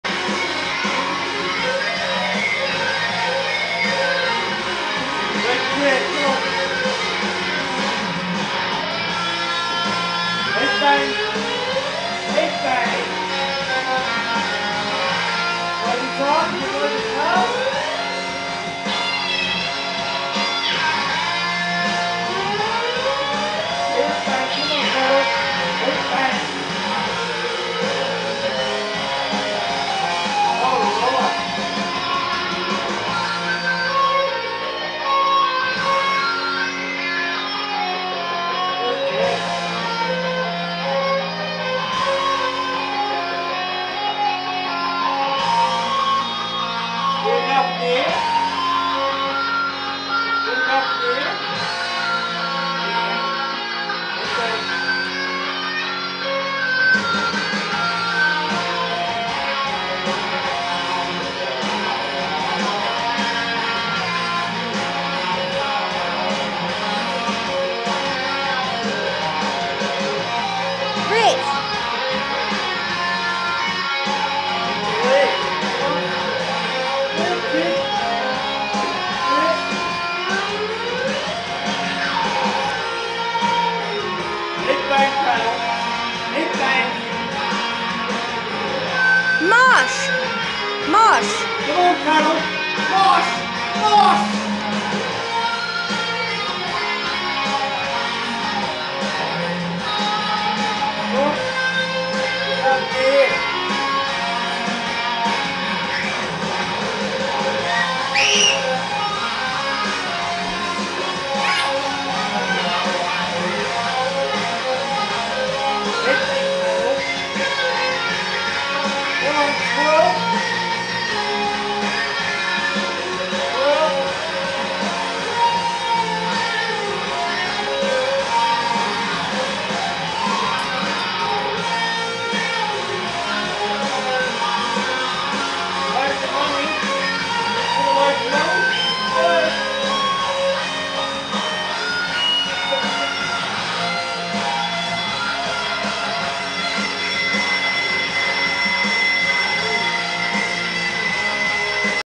Daddy and Skye Dance Lesson 16 July 2009
Rich was teaching her to Mosh, Headbang etc you name it! she loved it.